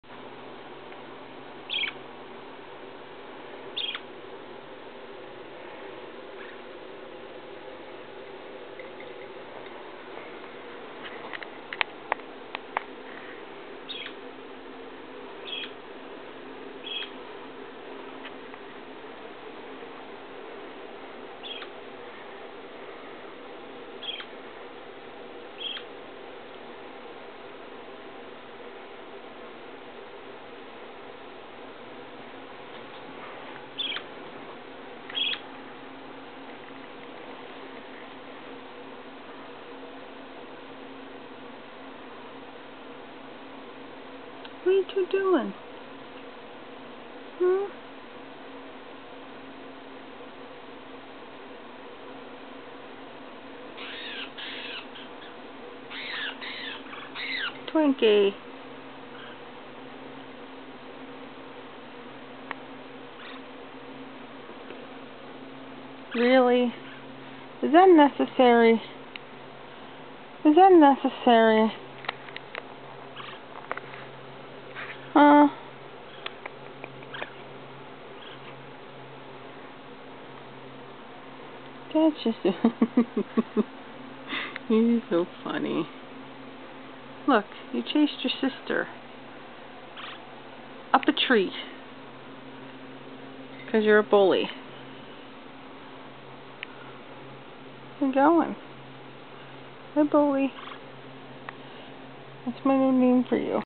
Twinkie being mean AGAIN.avi
The girls are sitting all nice and pretty then Twinkie loses it!  But watch her waddle...it is to funny!